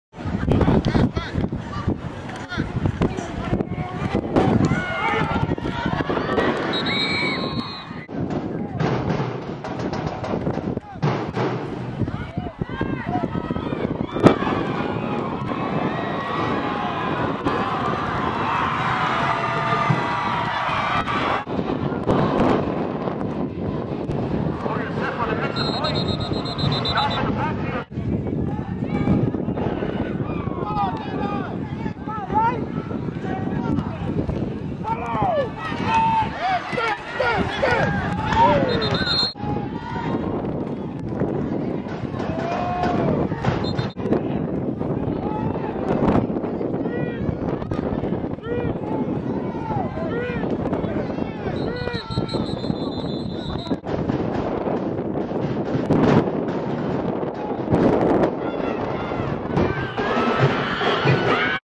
Tafuna Warriors 24 - Faga'itua Vikings 0 (Junior Varsity)
Samoa News brings you the Warriors and Vikings match up in the Junior Varsity Division, where the Warriors shut out the Vikings in their 24 point victory, as they maintain their No.2 ranking into the second round of the regular season.